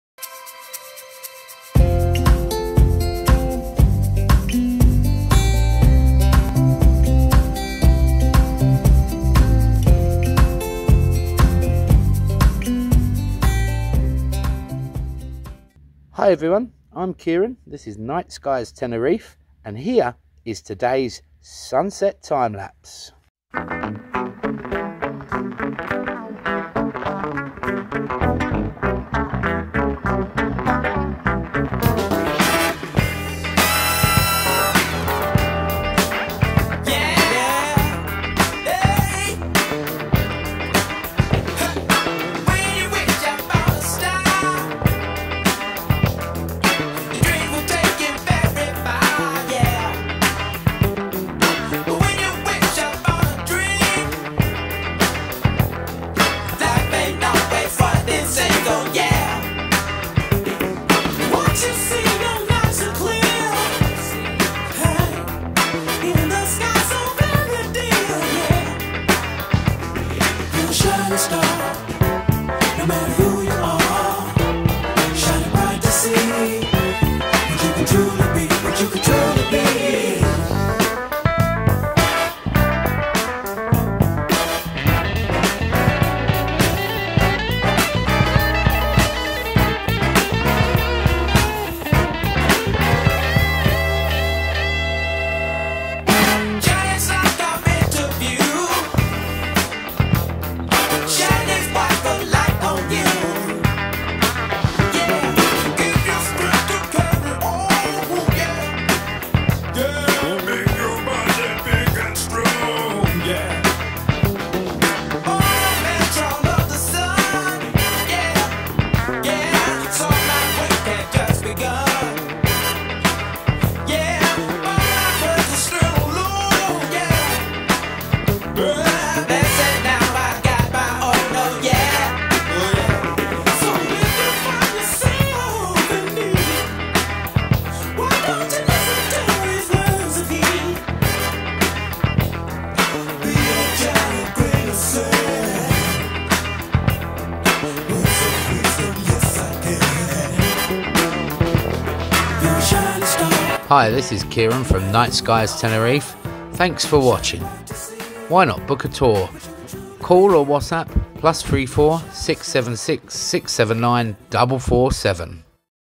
Sunset time-lapse 11th may 2021
Another evening with perfect skies for sunset and stargazing!  Enjoy last nights sunset in our video and as always a few photos from our star safari.